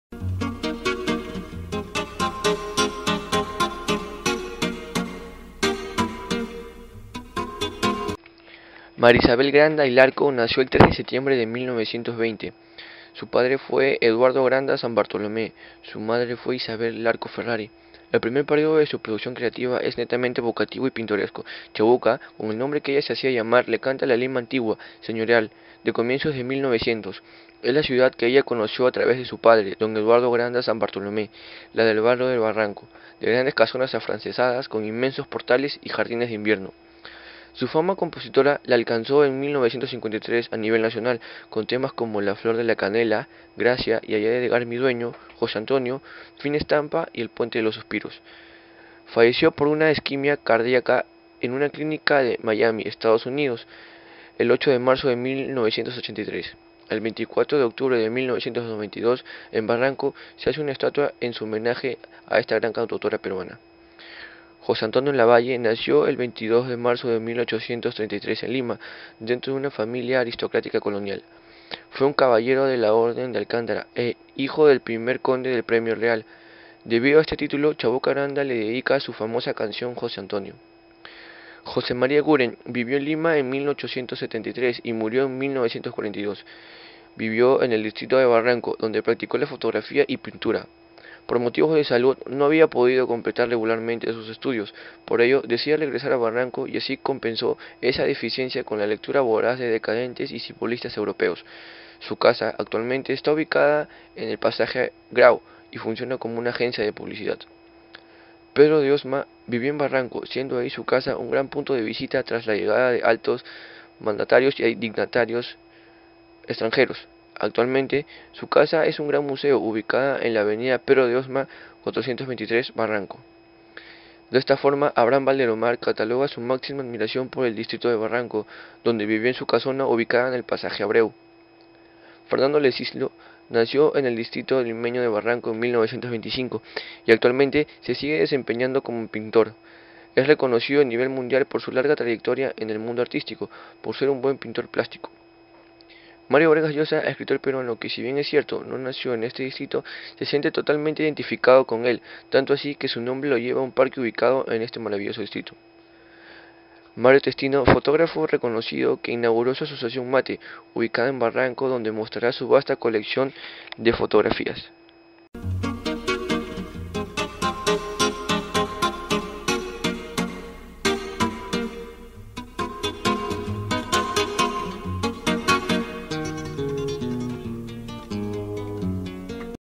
PERSONAJES DE BARRANCO
en este vídeo hacemos una pequeña reseña acerca de los personajes mas importantes de Barranco

video hecho por: Arturo Bustinza